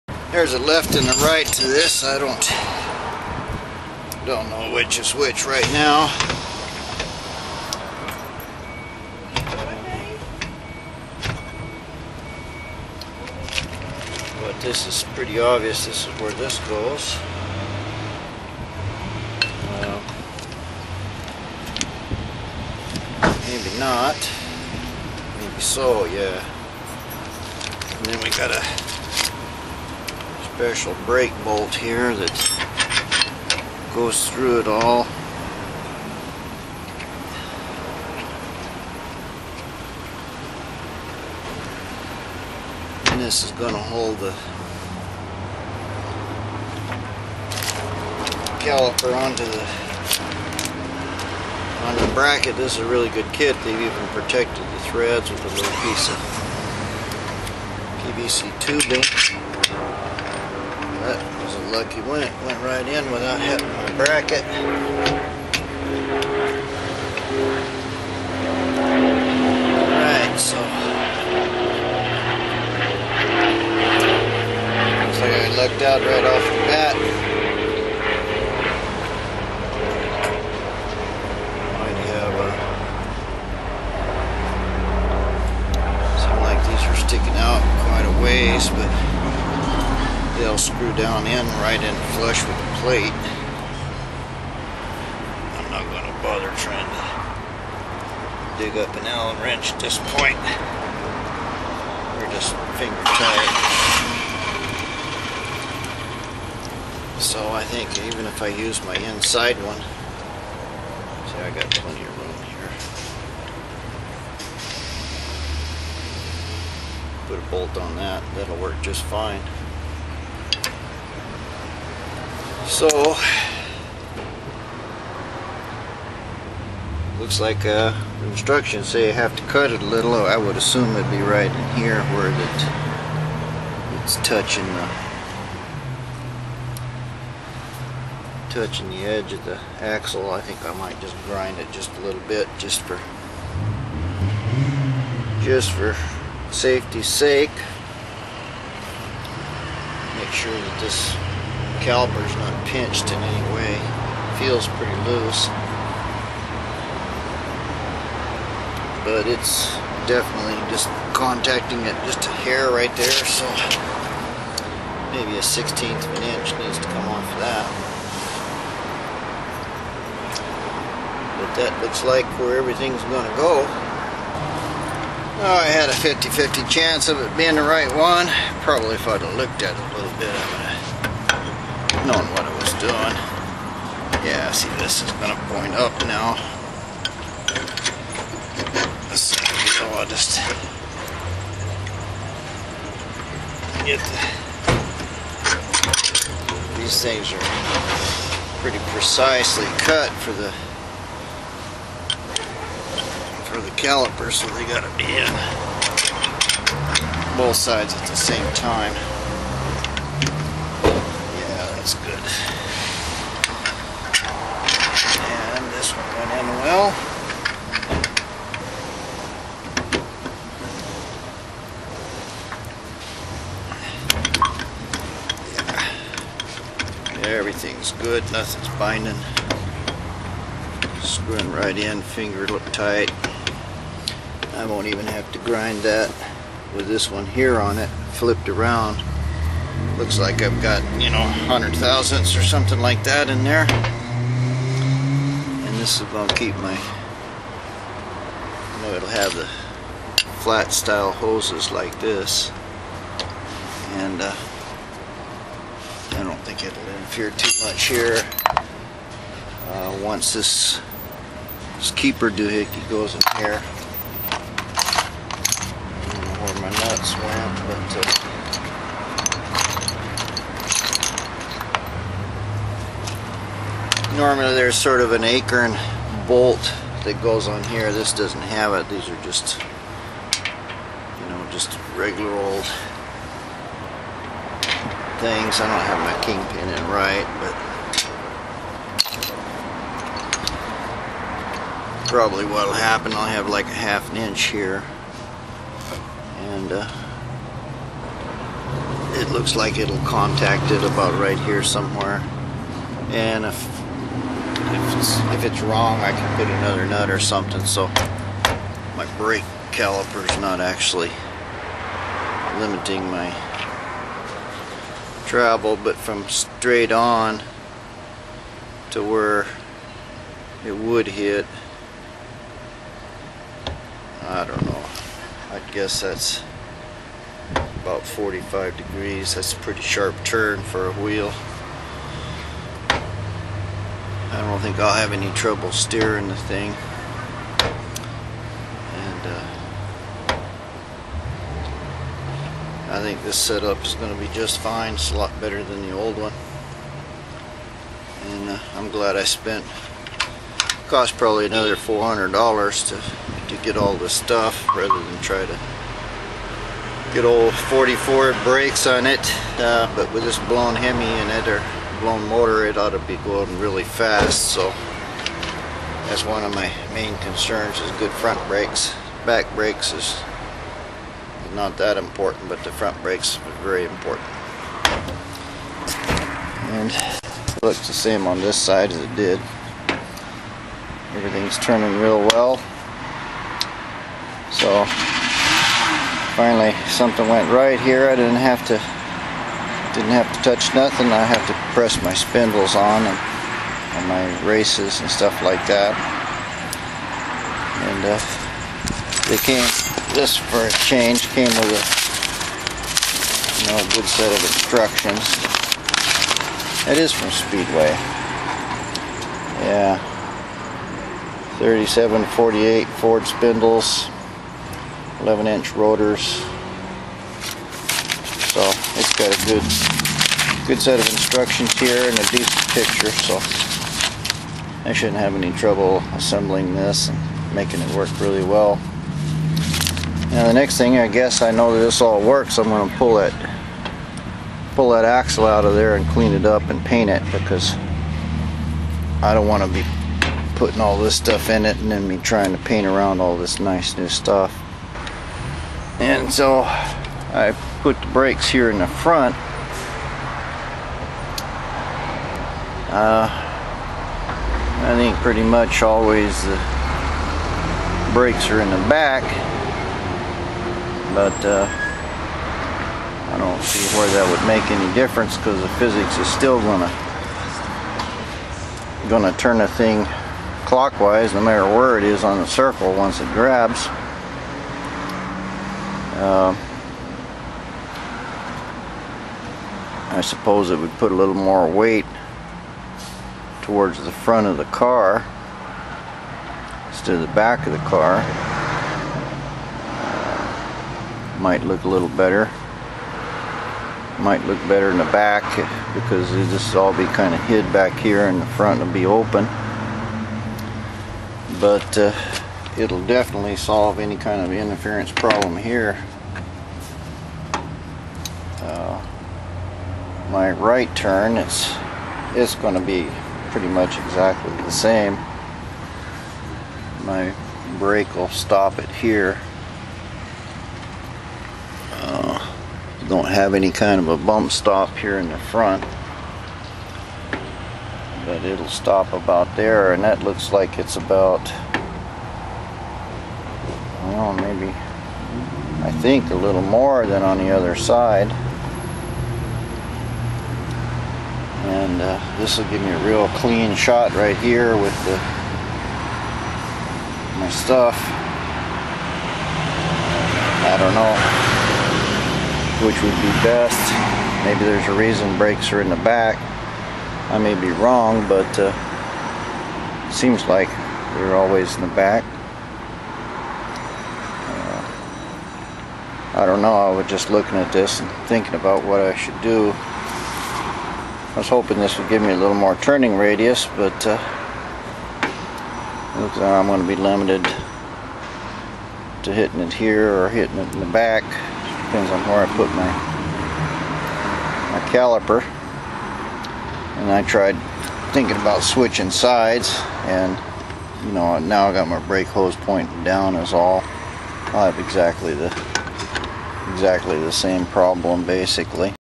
1932 Ford TBucket Installing speedway calipers on new king pins and straight axle
Installing speedway calipers on new king pins, working out right and left, determining steering angles.